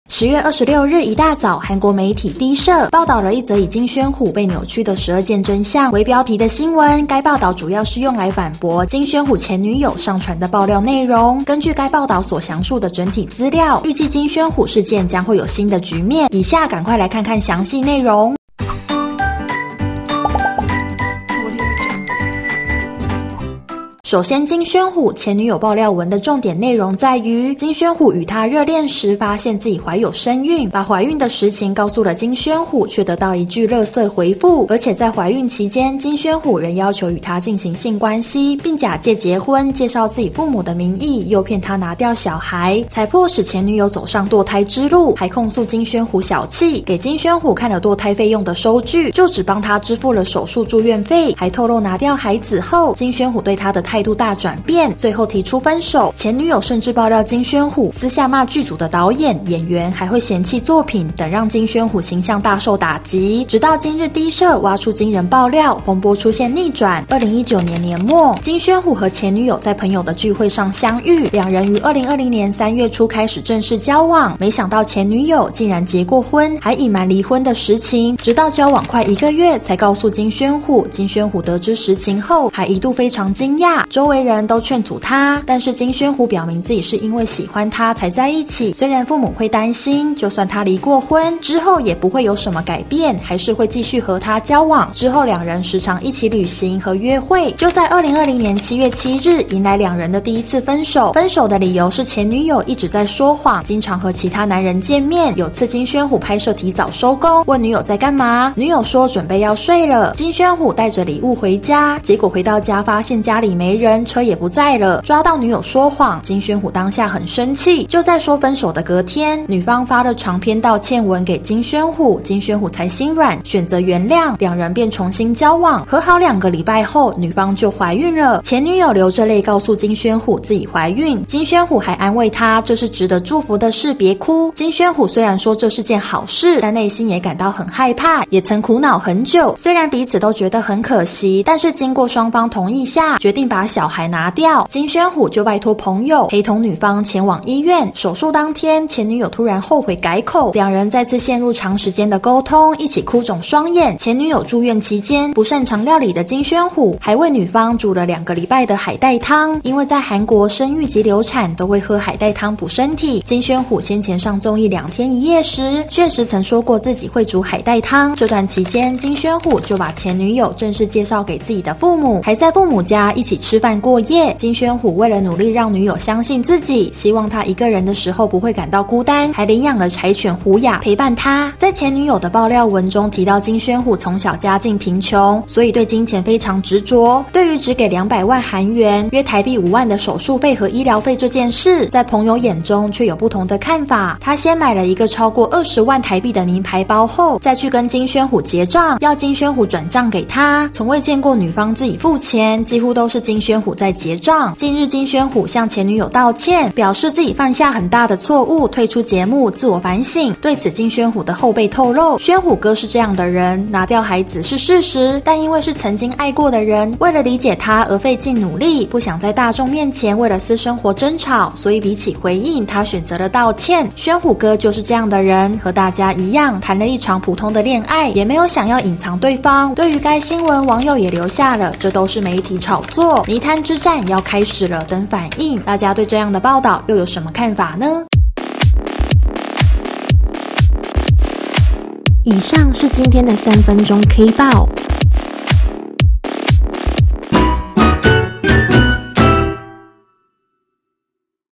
D社爆料金宣虎事件大反轉，12件真相曝光
10月26日一大早韓國媒體D社報導了一則以「金宣虎，被扭曲的12件真相」為標題的新聞，該報導主要是用來反駁金宣虎前女友上傳的爆料內容，根據該報導所詳述的整體資料，預計金宣虎事件將會有新的局面？以下趕快來看看詳細內容…